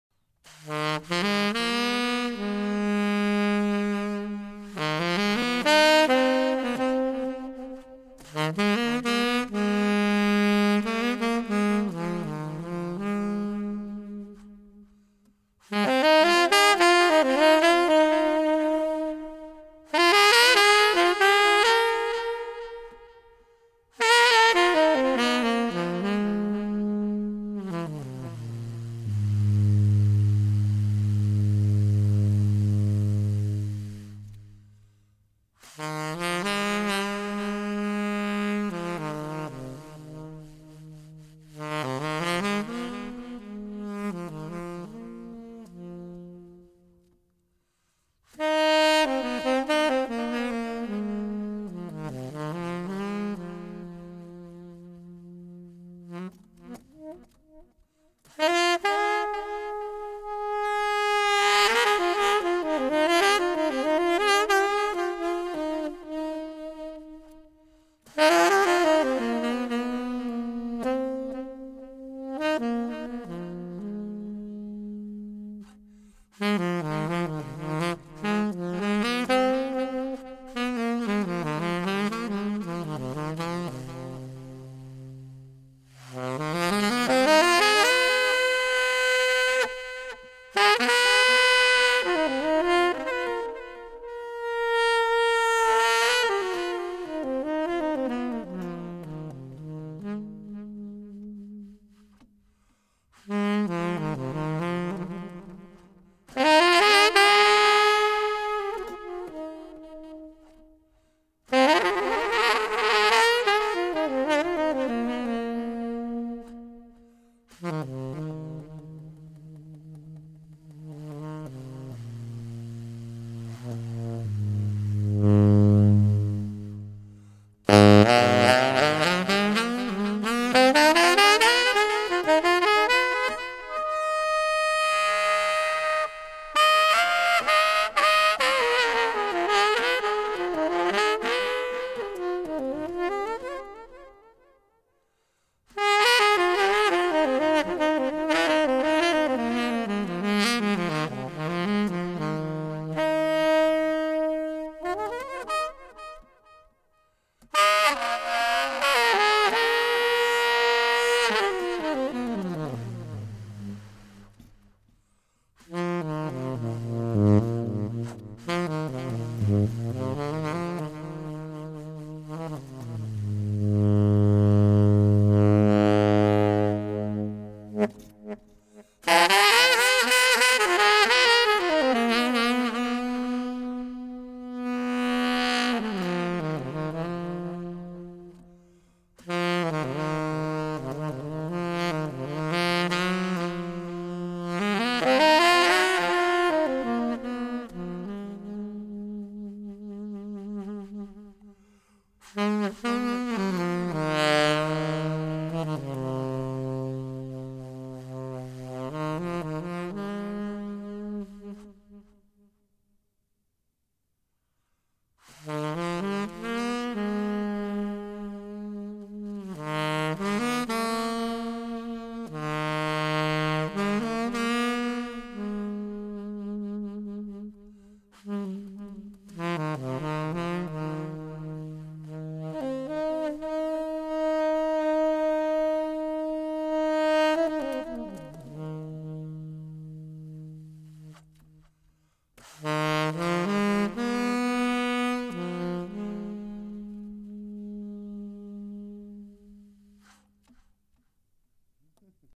Chillin Sax Solo 😎🎷
Just me soloing for a relaxed feel. Hope you can sit back and have a listen for the subtle dynamics and techniques. It's all about the sax for this one.